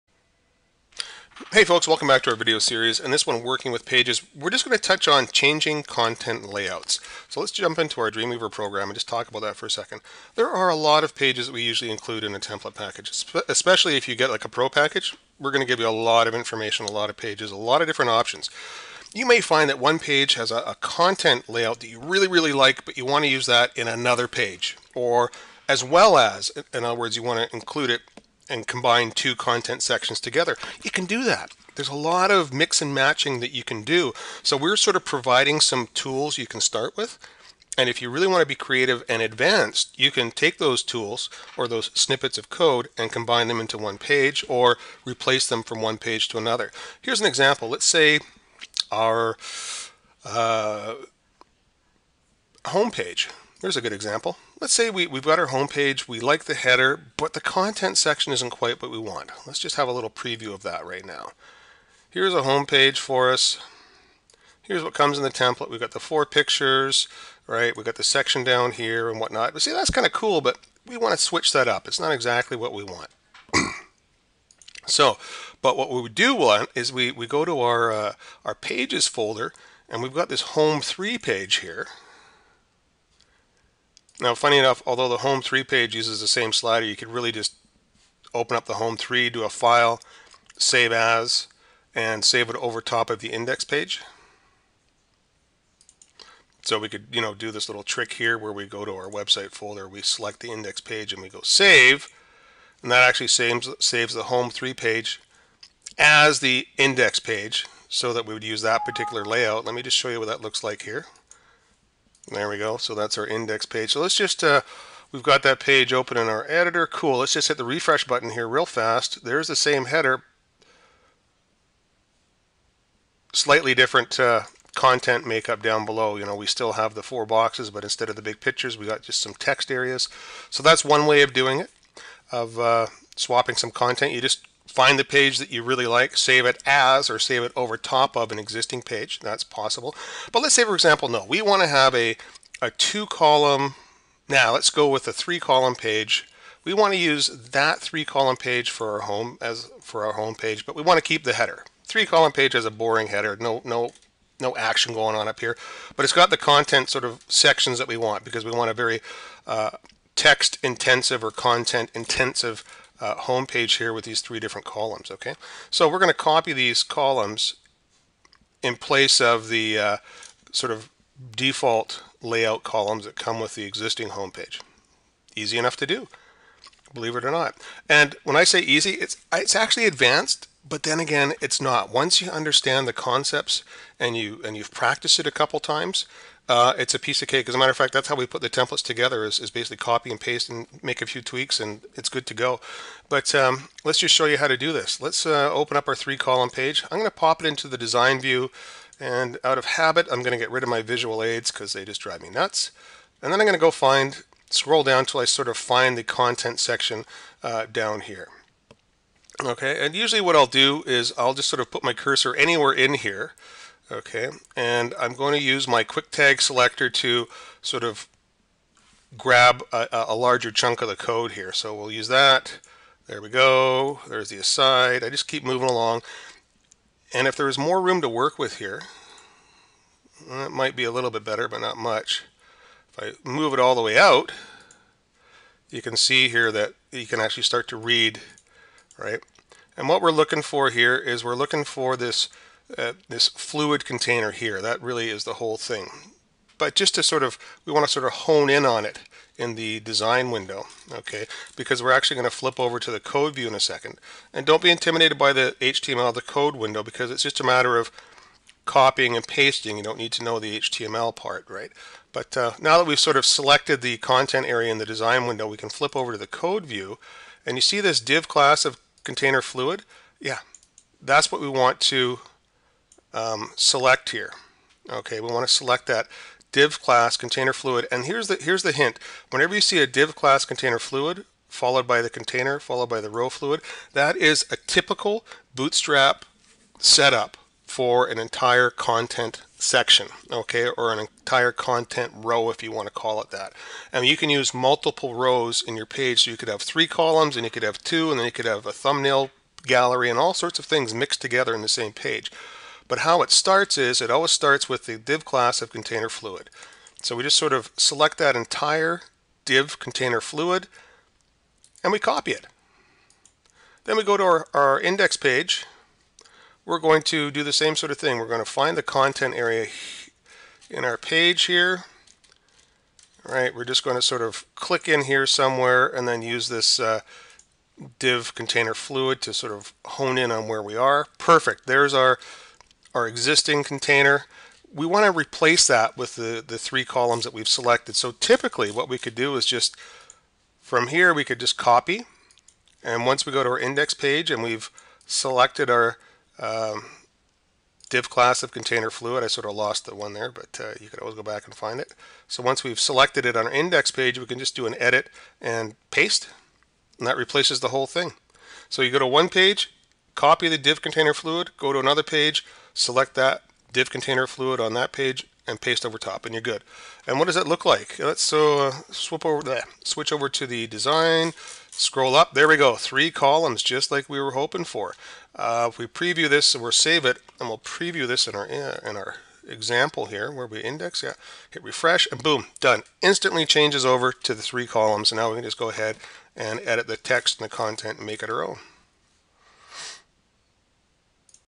Chaning content layouts Dreamweaver
Learn how to change content layouts for your Bootstrap Template using Dreamweaver.